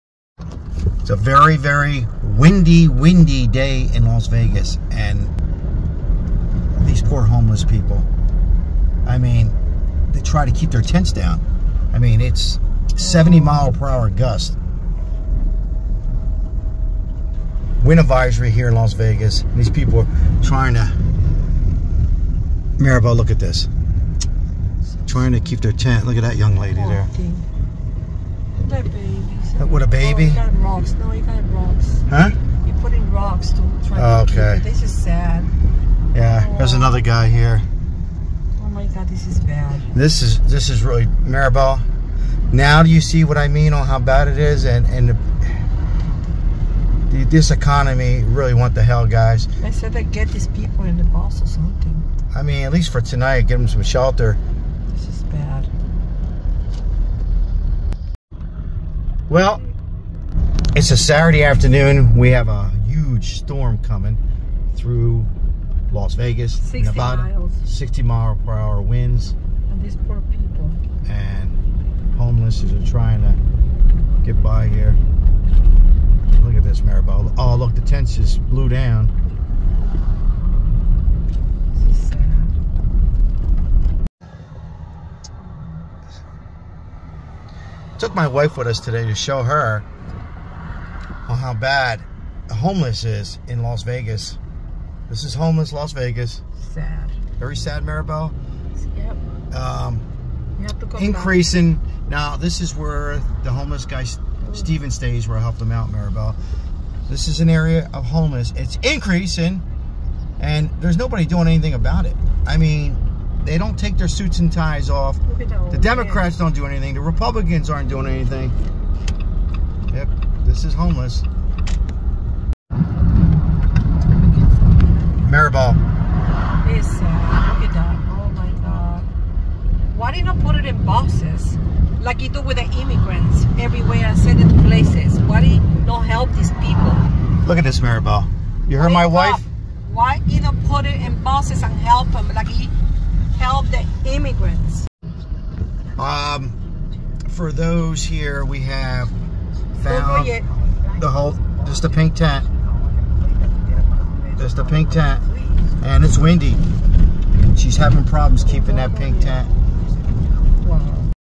HOMELESS LAS VEGAS in MASSIVE STORM
Homeless Las Vegas buckle down there tents from this huge Nevada Wind storm.